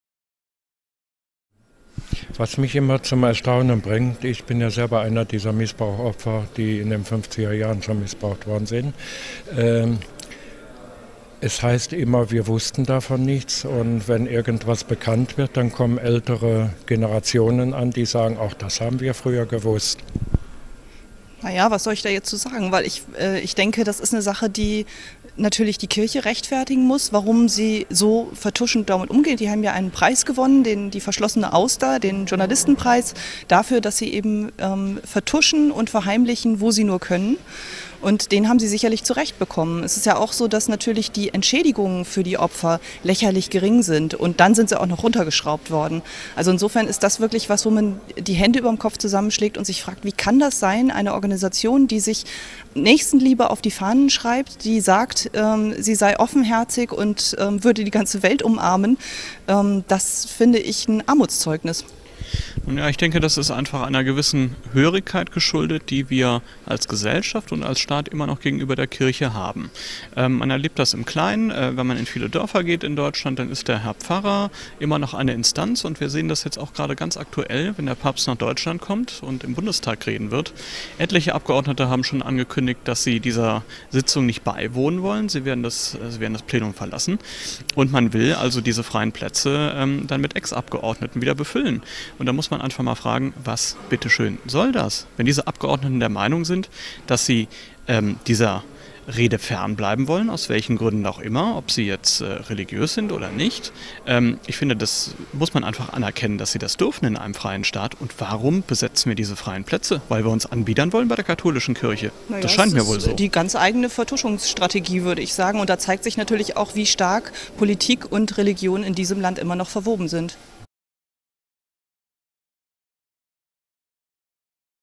Interview_Vertuschung der Kirche.mp4
Missbrauchvertuschung durch die Kirche
Anne Weiss und Stefan Bonner die mit Ihren Buch "Heilige Scheiße" Ahnungslose und Erleuchtete unter die Lupe nehmen, stellten fest: Die kath. Kirche hat schon immer versucht die Missbrauchtaten in Ihren eigenen Reihen zu vertuschen, statt sinnvoll aufzuklären